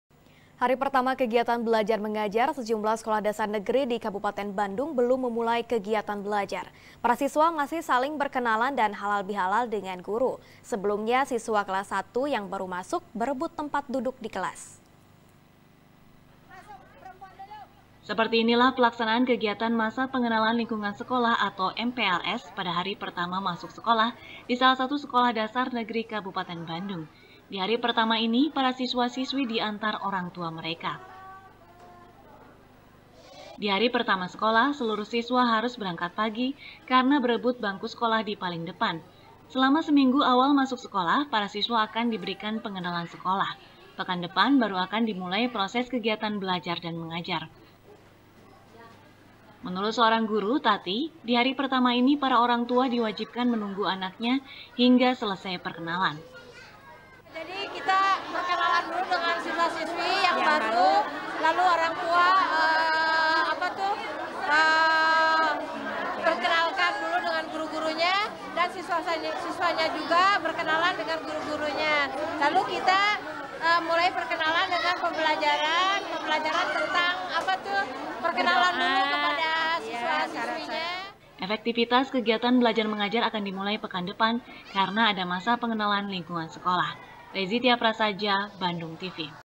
Hari Pertama Sekolah, Siswa Berebut Tempat Duduk | SBR | BANDUNG TV
SBR | BANDUNG TV | DINDA RIZKYANA | Hari pertama kegiatan belajar mengajar, sejumlah sekolah dasar negeri di kabupaten bandung, belum memulai kegiatan belajar. para siswa masih saling berkenalan dan halal bilhalal dengan guru. sebelumnya, siswa kelas satu yang baru masuk, berebut tempat duduk di kelas.